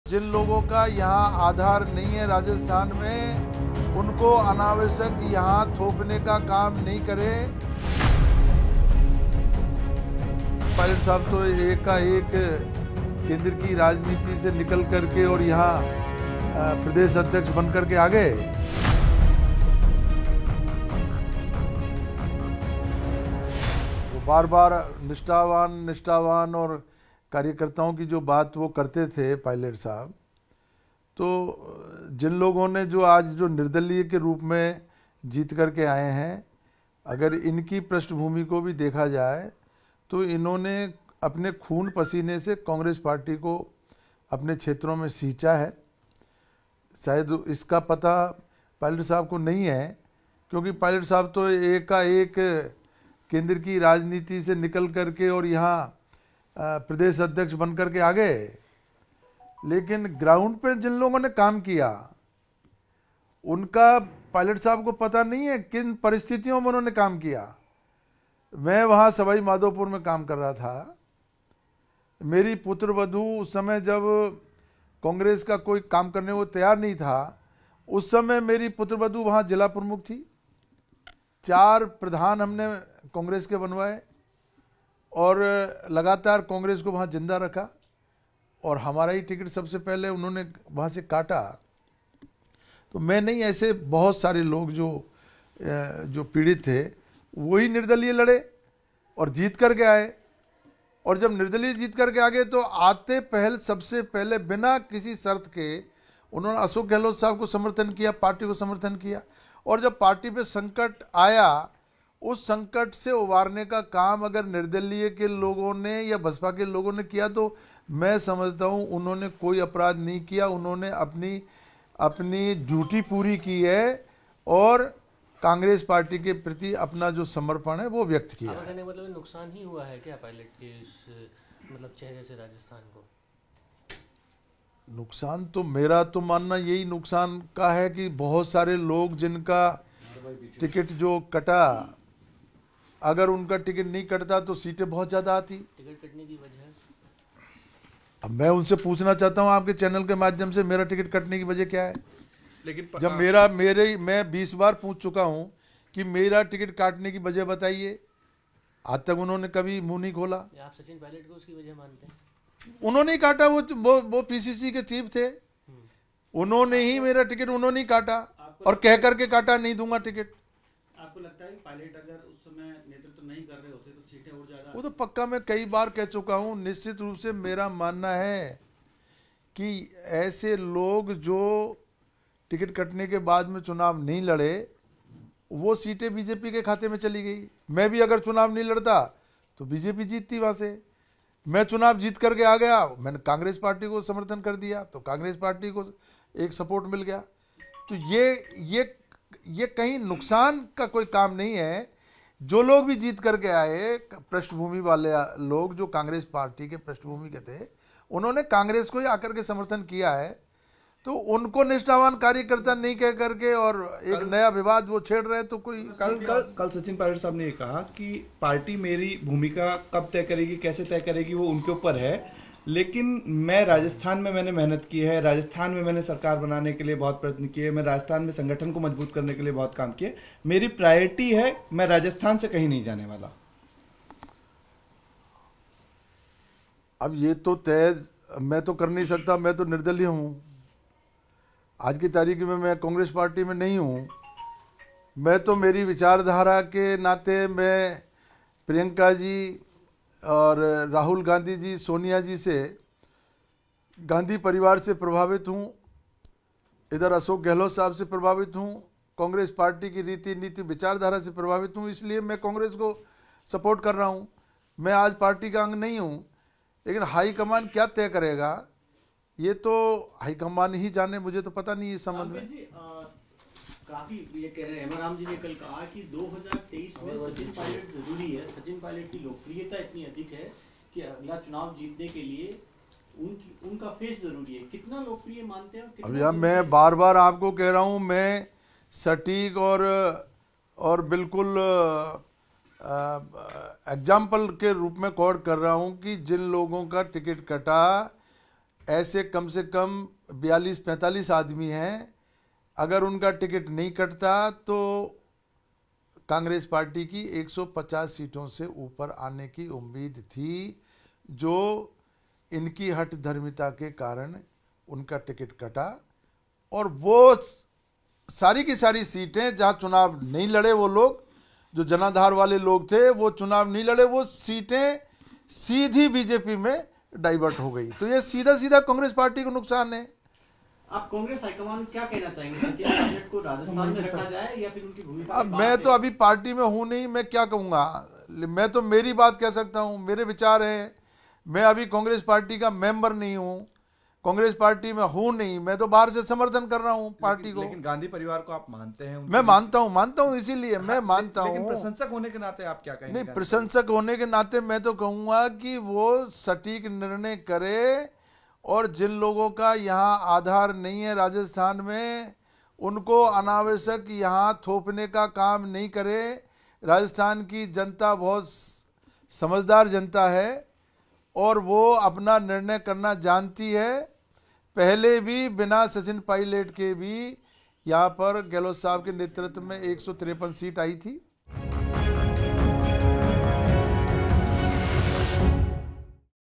Ashok Gehlot के सलाहकार रामकेश मीणा का सचिन पायलट पर हमला, बोले पायलट की वजह से पार्टी को हुआ नुकसान
मुख्यमंत्री के नवनियुक्त सलाहकार रामकेश मीणा ने सचिन पायलट पर हमला बोलते हुए कहा कि सचिन पायलट ने  पार्टी का बंटाधार कर दिया है. उन्होंने कहा कि अगर पायलट नहीं होते तो कांग्रेस की आती 150 से अधिक सीटें आती.